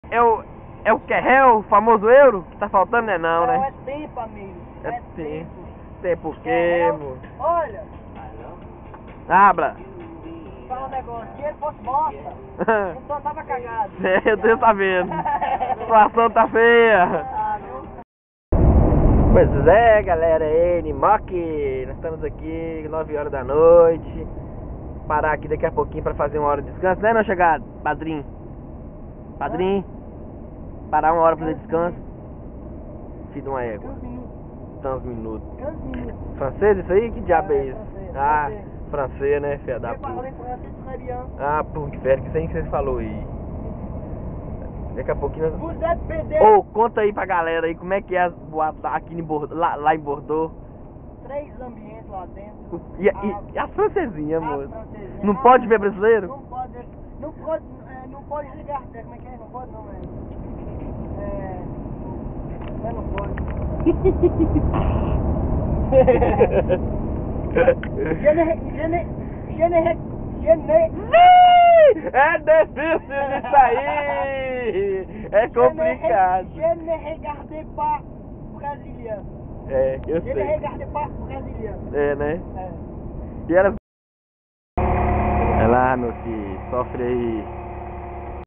Fabiano engasgando no frances.
eu e fabiano pela estrada, e ele dando aquela tipica engasgada na hora de falar o frances. so pra rir.